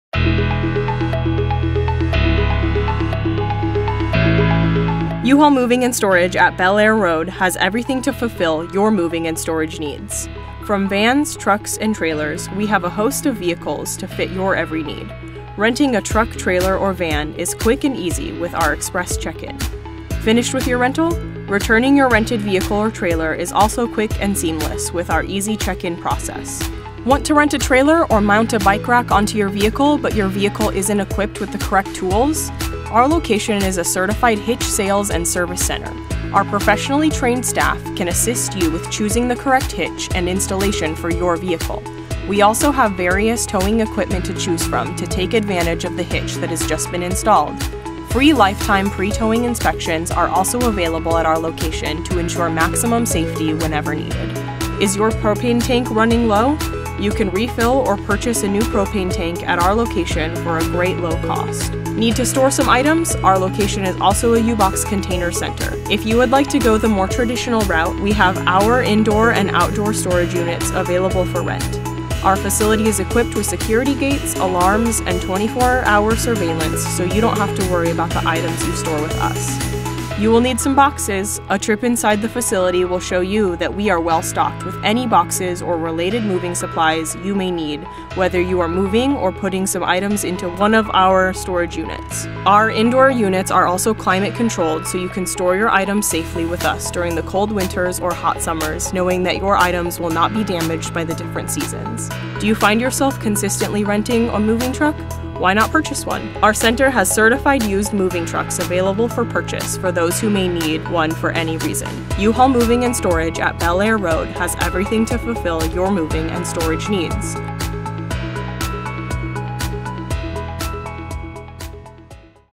U-Haul Moving & Storage at Belair Road
U-Haul Moving & Storage at Belair Road has everything to fulfill your moving and storage needs. Whether you're looking to rent a truck or a trailer for your move or place your RV, boat, motorcycle, personal vehicle or other personal belongings in storage, U-Haul Moving & Storage at Belair Road has got you covered. Check out some of the features this store has to offer.